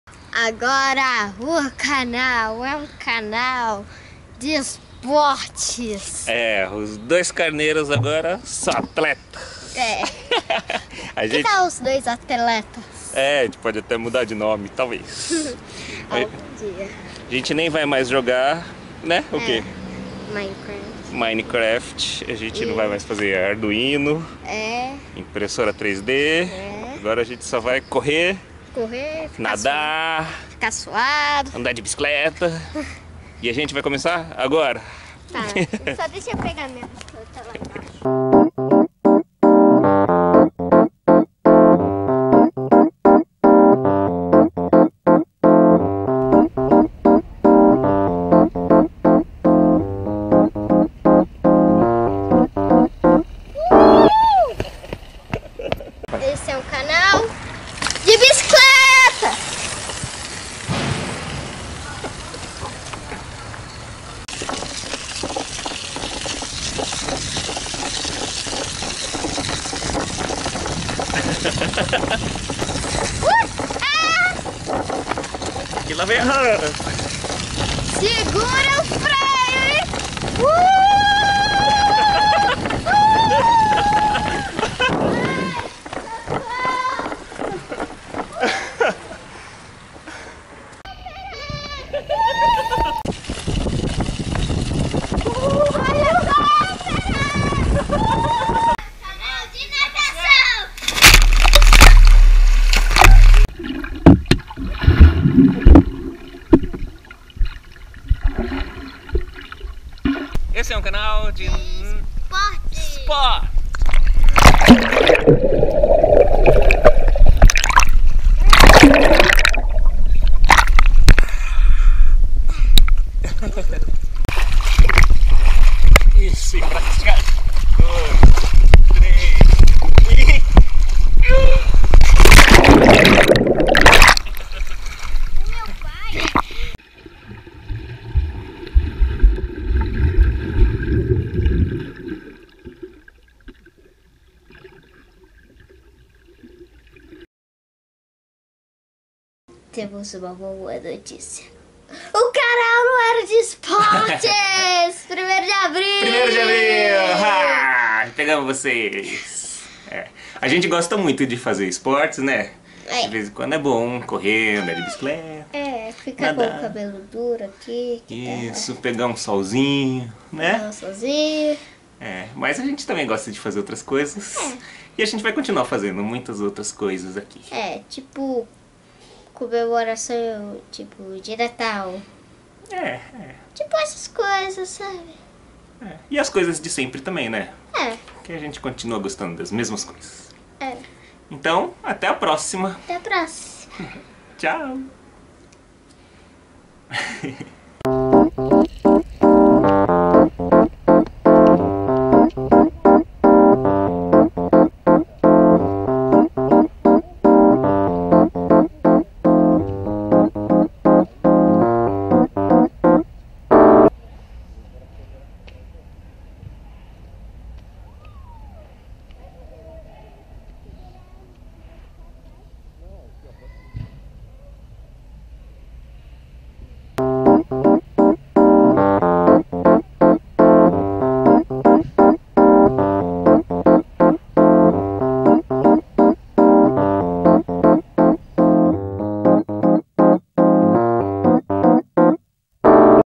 2Carneiros - Esportes!
Os 2Carneiros mostram o novo tema do canal: Esportes!! Bicicleta, corrida e natação! Nada de arduino, nada de Minecraft, nada de jogos nem impressora 3D. Nunca mais. Será?!!